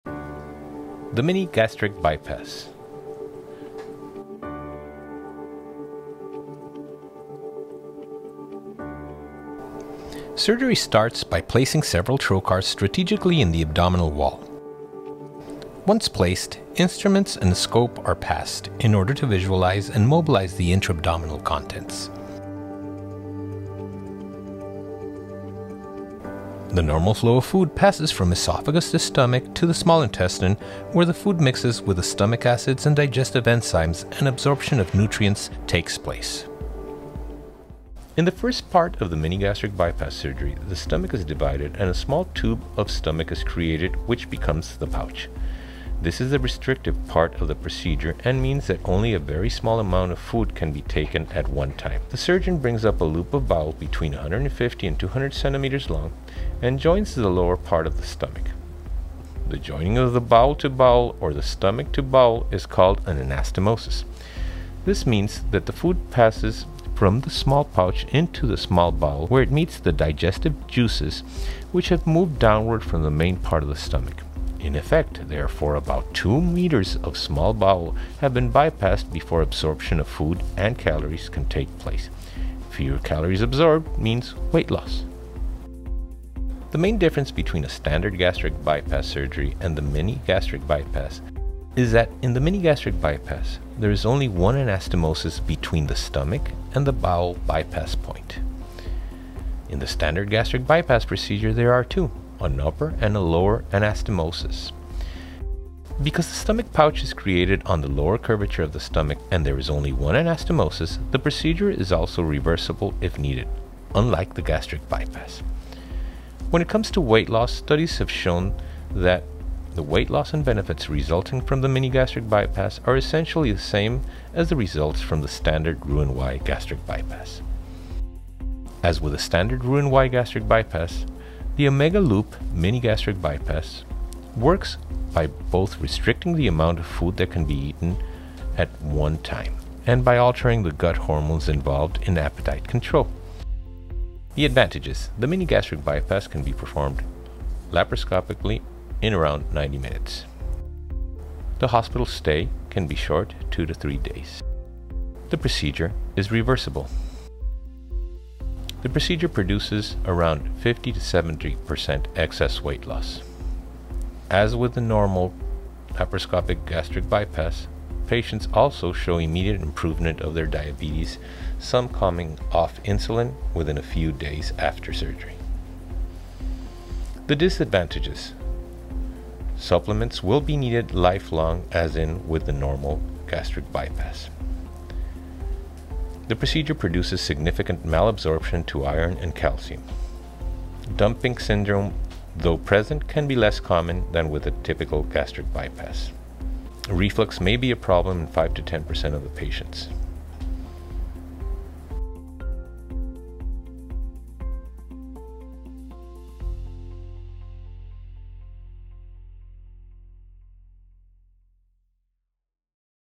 Mini Gastric Bypass Explained by Master Surgeon Dr. Ariel Ortiz
In this video, our master bariatric surgeon, Dr. Ariel Ortiz, breaks down the mini-gastric bypass. He shows us the science behind how the procedure is performed and how it results in weight loss. Additionally, he highlights the pros and cons to help you make an informed decision on which form of bariatric surgery would best help you achieve your weight loss goals.

The mini-gastric bypass procedure consists of creating a smaller stomach pouch and attaching the terminal part of the small intestine to the new stomach. The body's normal digestive process is altered, restricting food intake and the number of calories the body absorbs. As a result, food bypasses a large part of the stomach and most of the small intestine. It is great for revising a failed sleeve or other failed procedure.

To read more about our Improved Gastric Sleeve, visit our